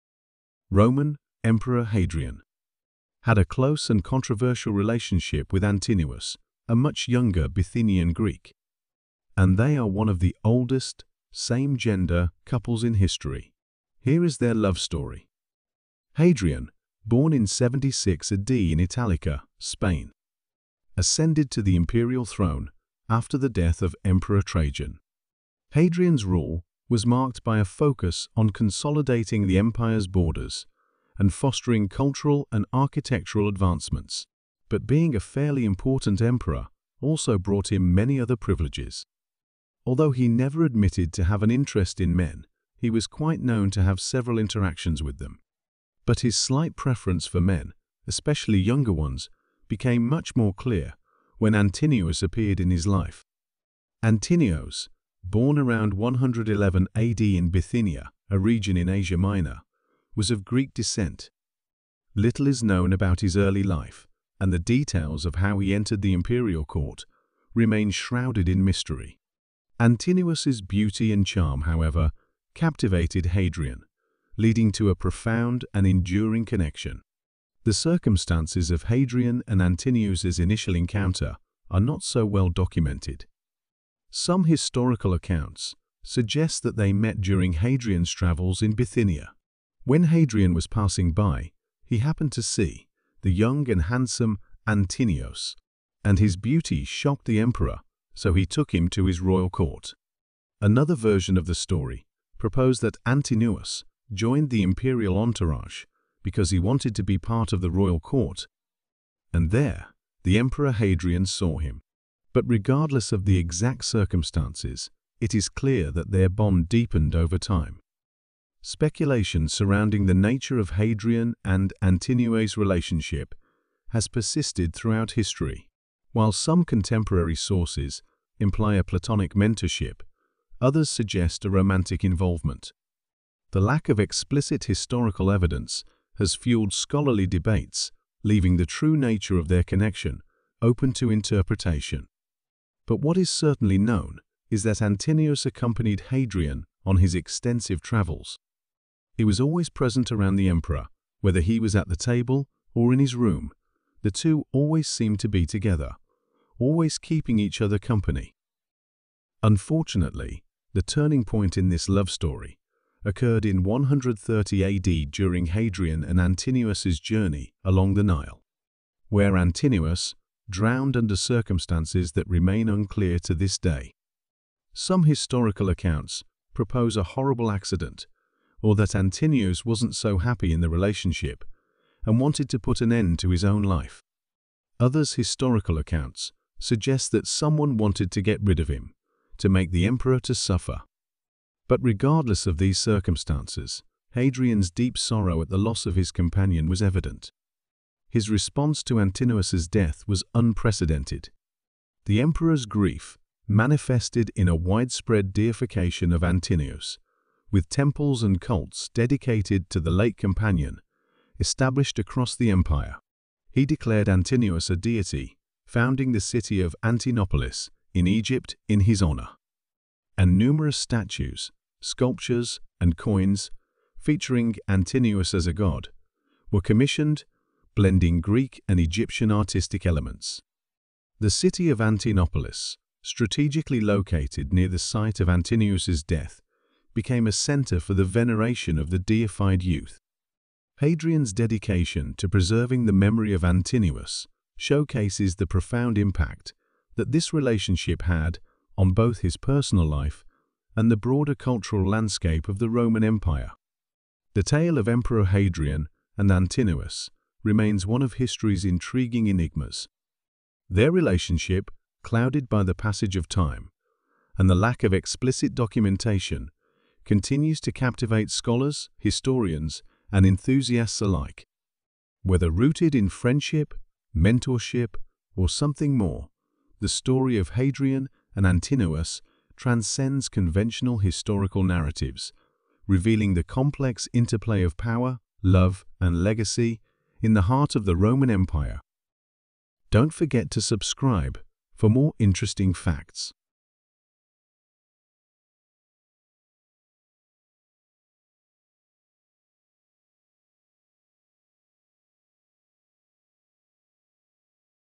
Emperor Hadrian and Antinous's love story
”Roman Emperor Hadrian and Antinous's love story”  is a small documentary about the old and tragic story between the Emperor Hadrian and the Greek boy Antinous.